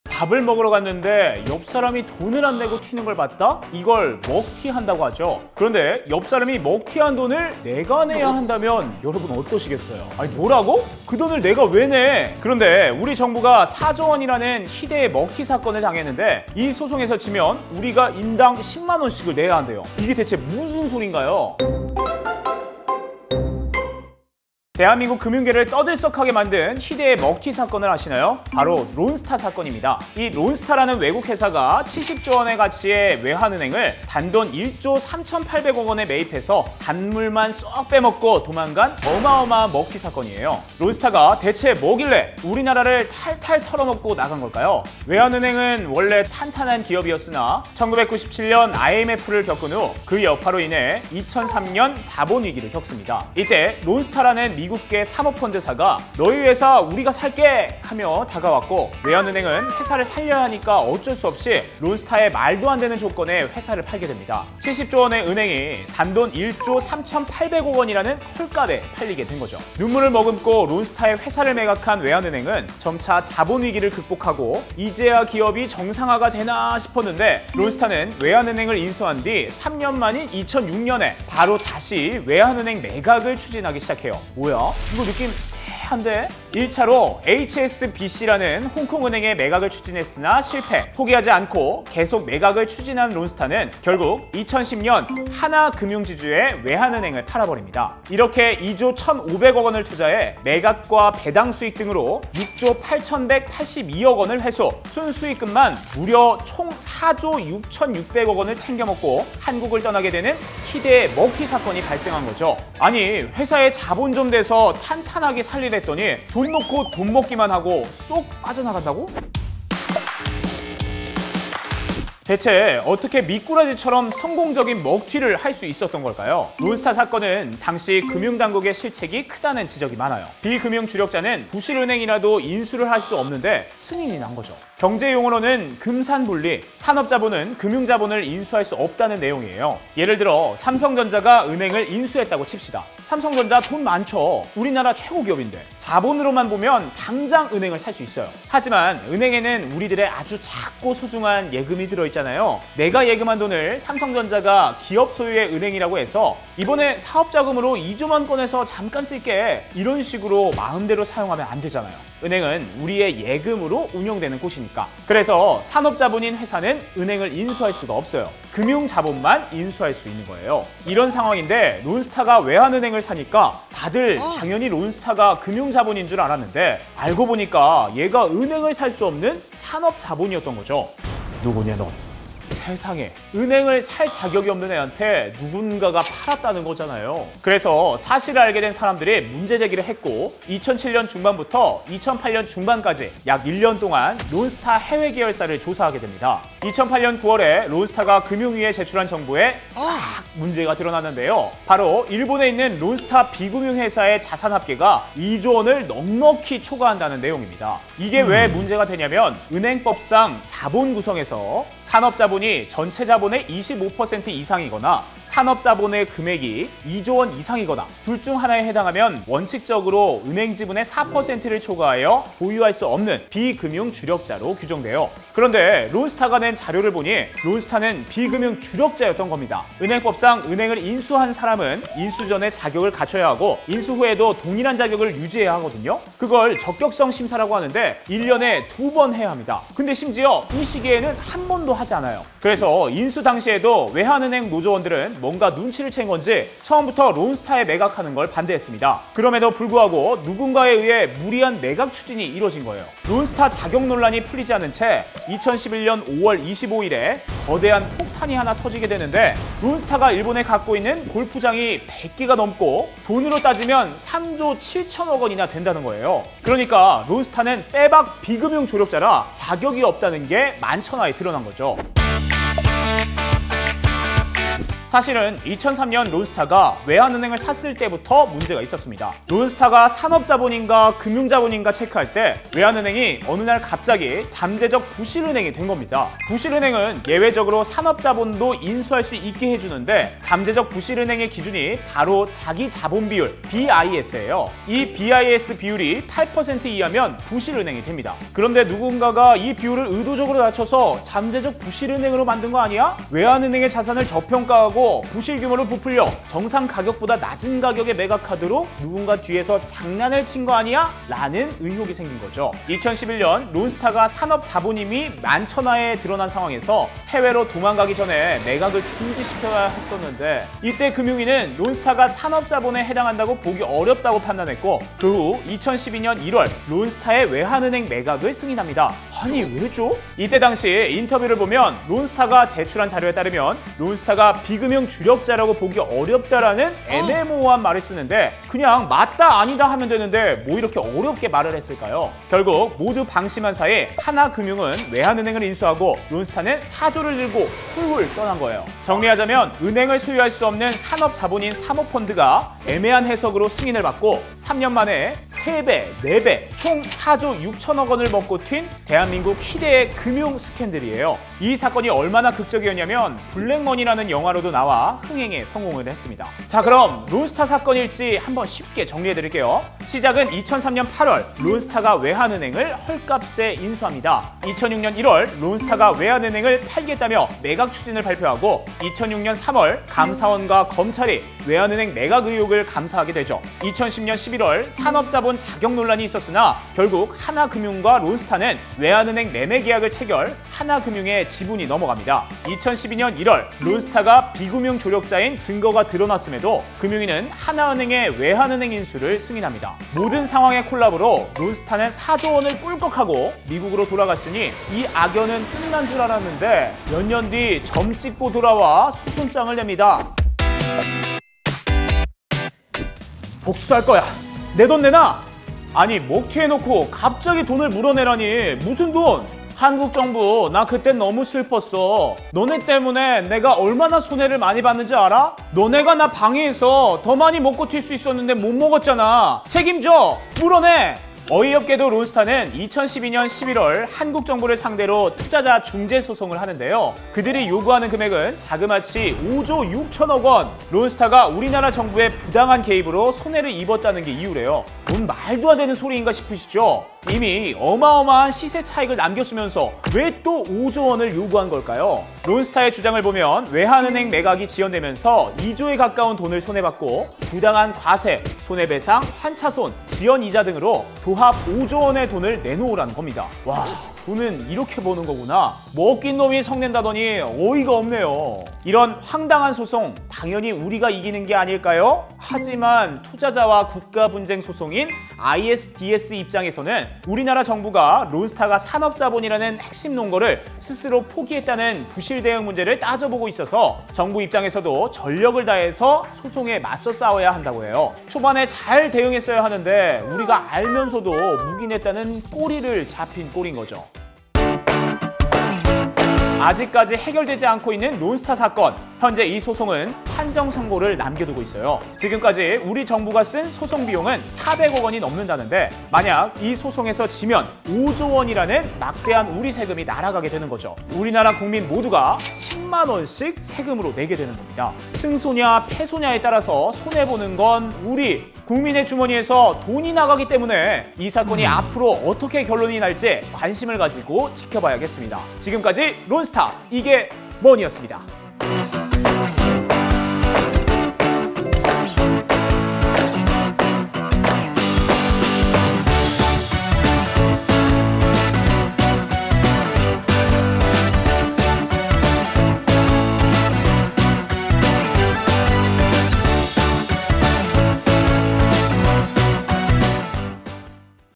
아까운 세금만 탈탈 털릴 각｜한국에서 5조 먹고 5조 더ㅣ론스타 총정리 [이게머니]
대한민국 금융계를 떠들썩하게 만든
희대의 먹튀, ‘론스타 사건’

‘론스타’라는 외국 회사가 대한민국에서 5조 원을
먹고 튄 어마어마한 사건인데요!

어이없게도 먹튀는 우리가 당했는데 소송에서 지면
우리가 인당 10만원씩을 내야한답니다!
이게 대체 무슨 소리인가요?

론스타 이게머니?

00:00 4조 원 먹튀를 당하다
00:41 론스타 사건이 뭐야?
02:00 론스타는 어떻게 먹튀를 해냈을까?
04:33 계속 드러나는 론스타의 그림자
06:11 론스타 사건 일지
06:51 먹튀해 놓고 또 돈을 달라고?
08:17 국민 모두가 10만 원씩 내야한다고?

* 매주 금요일 오후 5시 업로드
* 머니 어시스턴스 : 고은천 아나운서